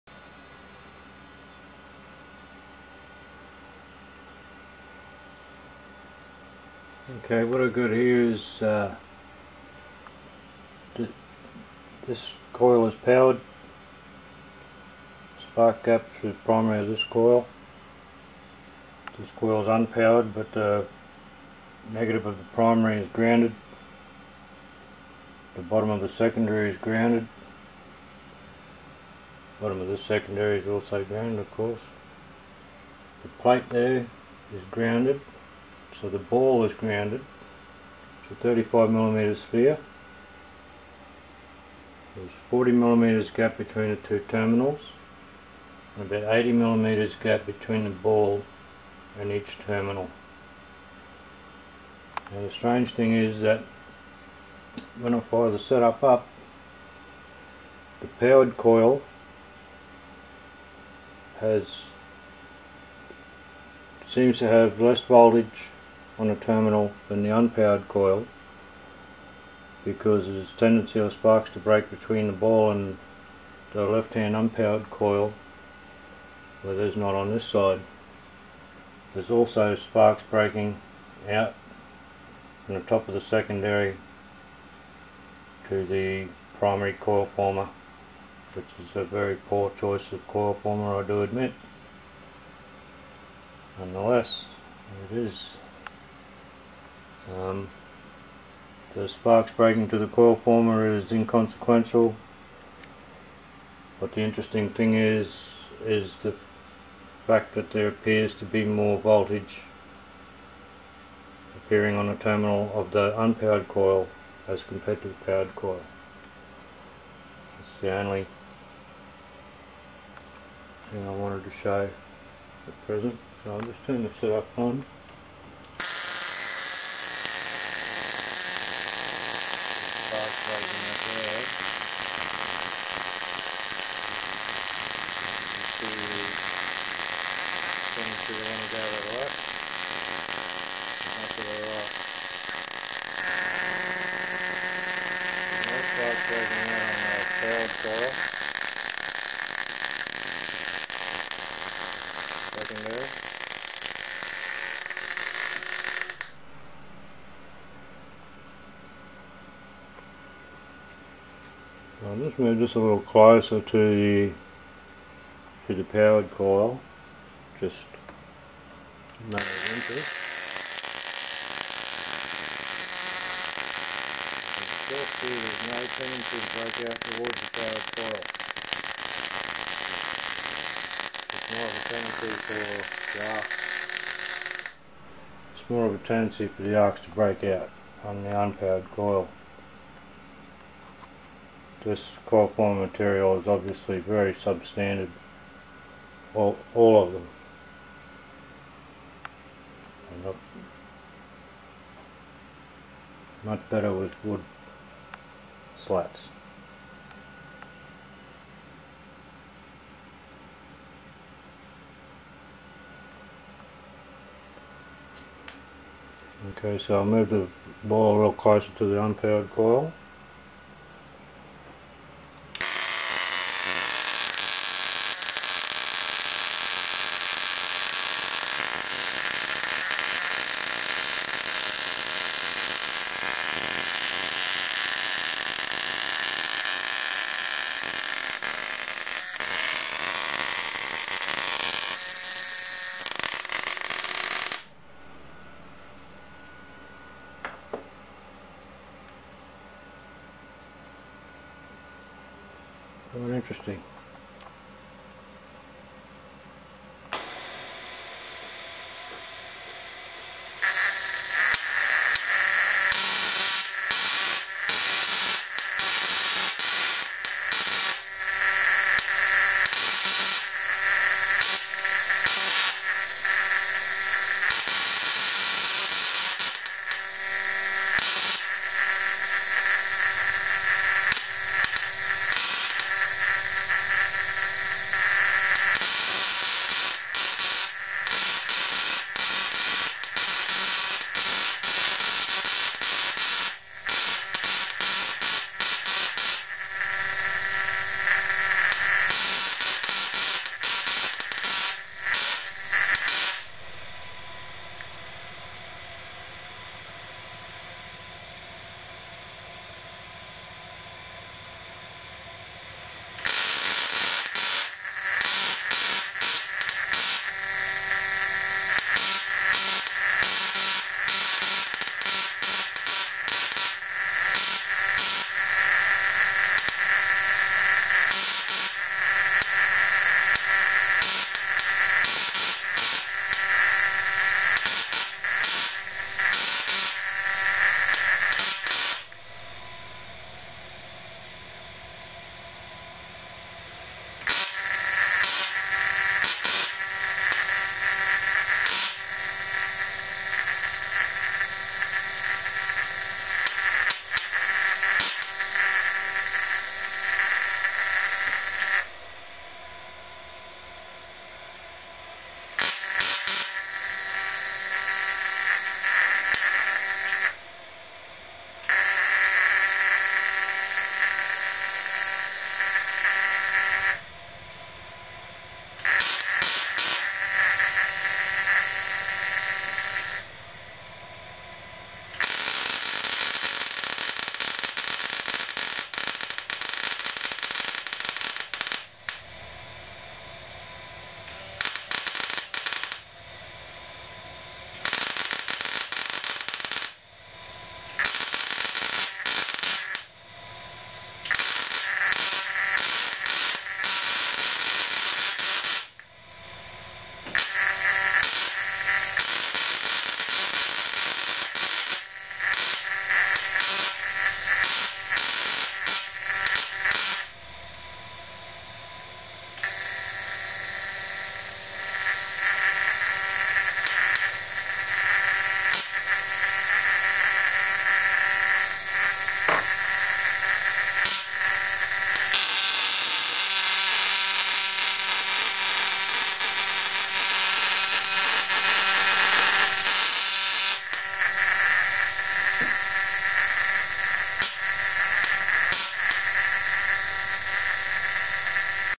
Effects 88.wmv
Extra voltage on second transformer (only wired connection to ground). Haven't worked out why yet. Second half is just testing stuff.